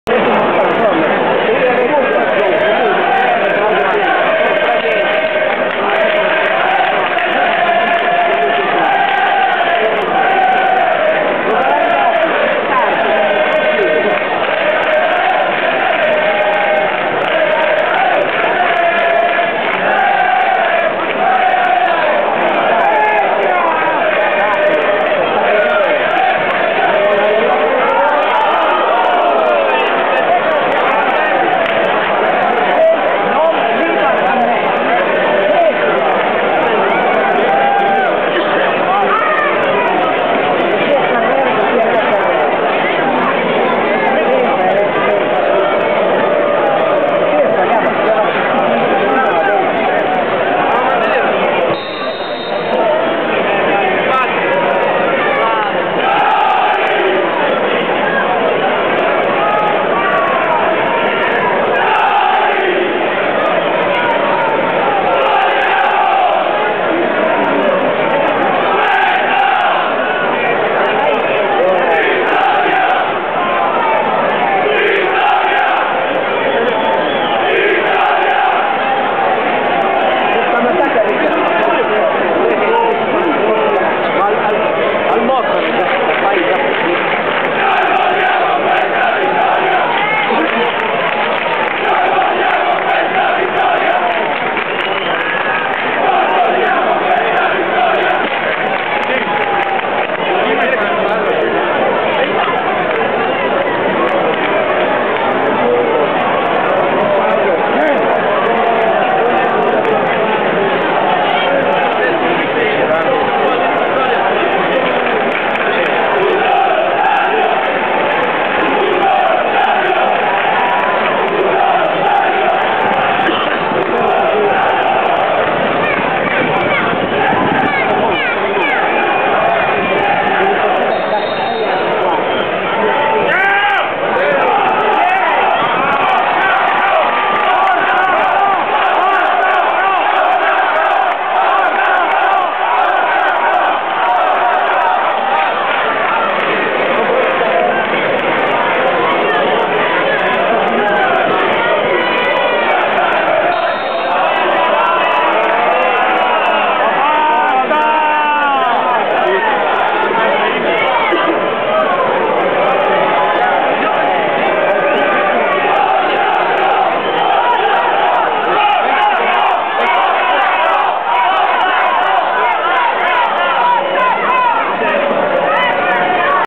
01 Pro Vercelli-Legnano 1-2
Il tifo della Curva Ovest animato dai "White Lions" - Serie C2 girone A 2006/07